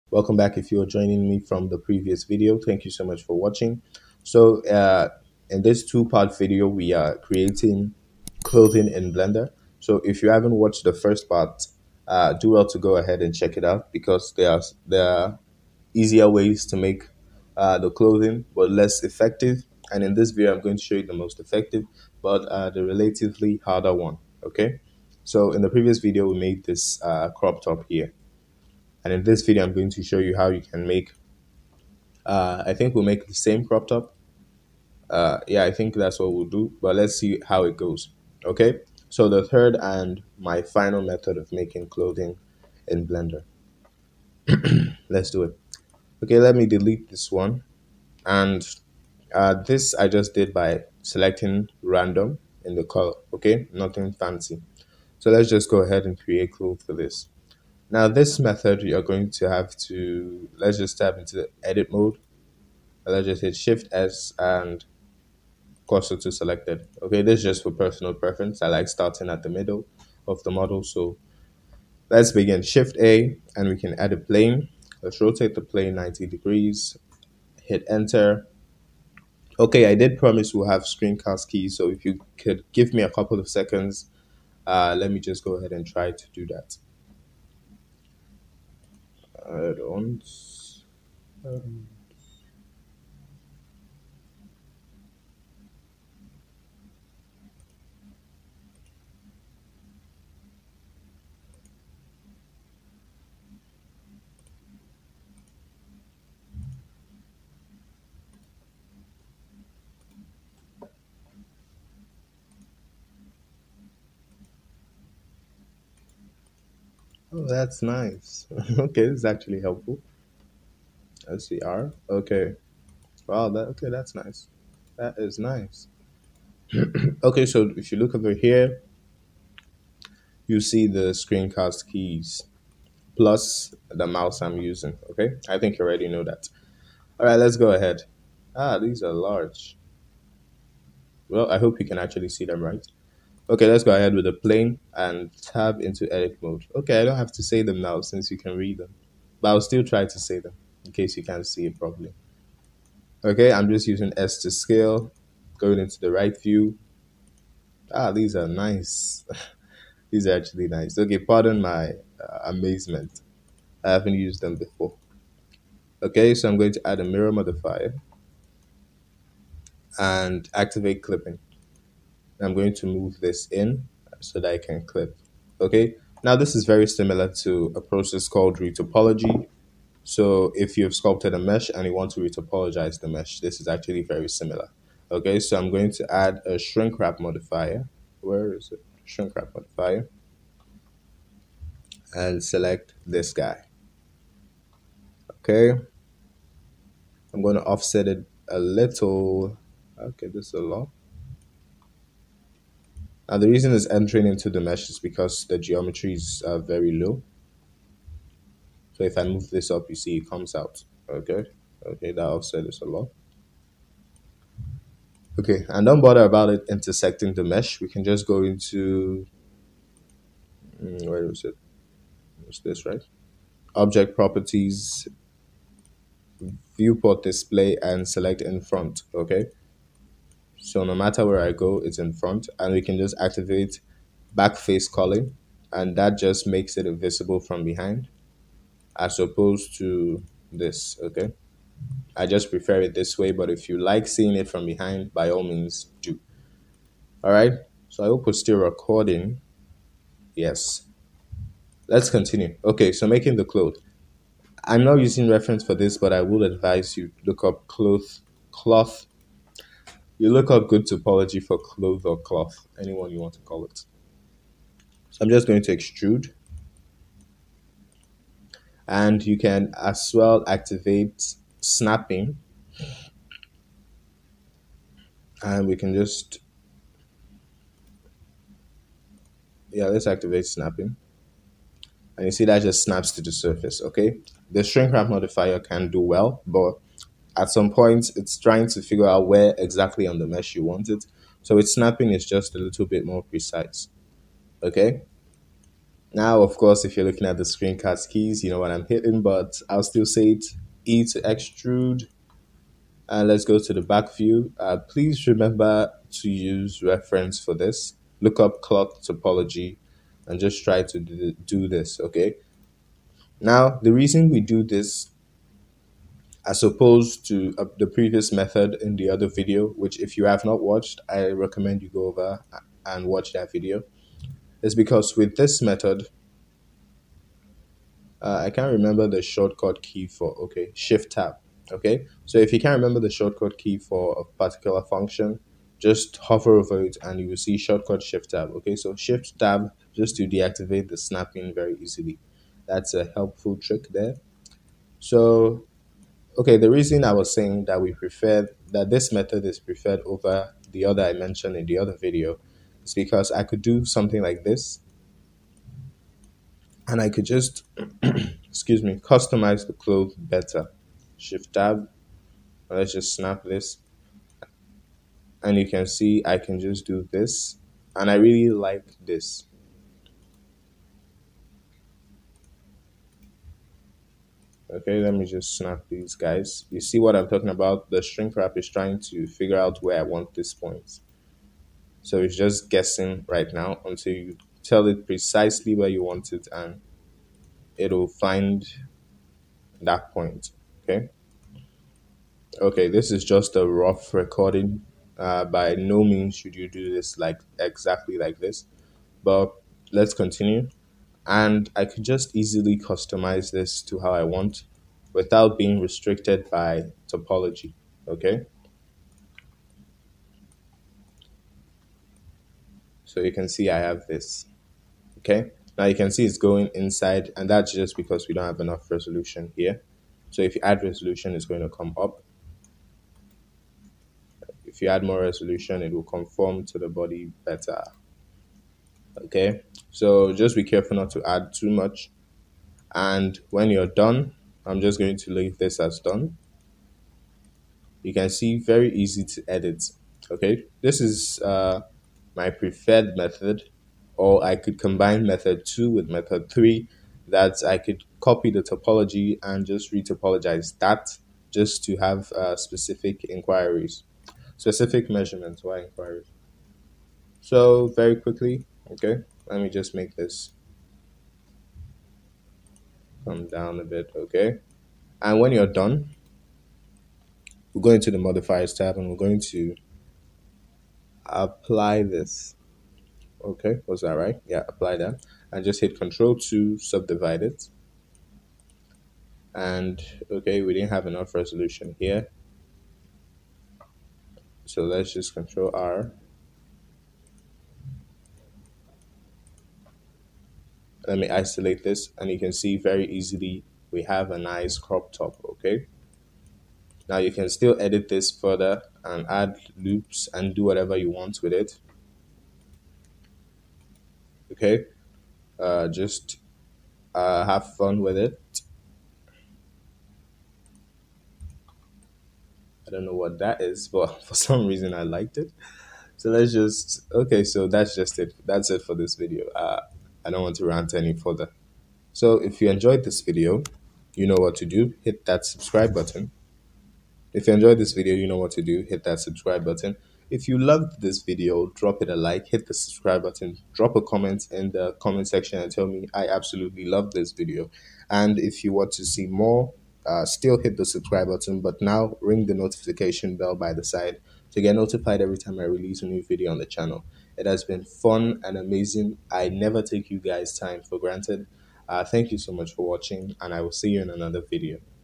How to Make Clothes In Blender 2.9 Part 2 | Beginners Tutorial
In this Blender Two part video series, I show how to make clothing in 3 different ways. No addons required for any of the three. Lets do it!!!
The images used for the thumbnail is from the Genesis 2 female for Daz Studio or poser created by Silver and WildDesigns.